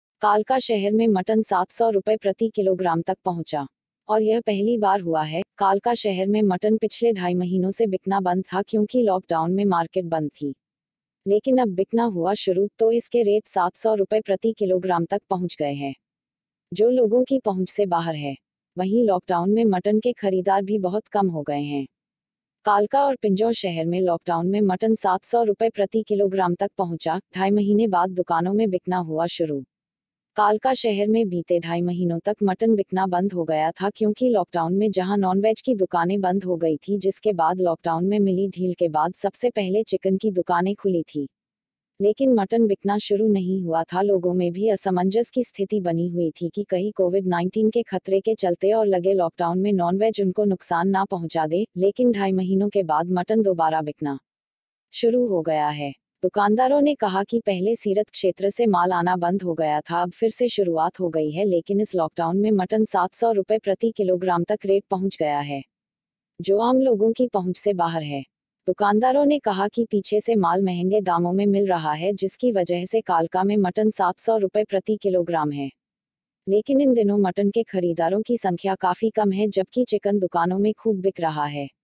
कालका में मटन इतना महंगा क्यों हुआ।
सिटी न्यूज।
     कालका शहर में पहली बार लॉक डाउन में मटन इतना मंहगा हुआ है।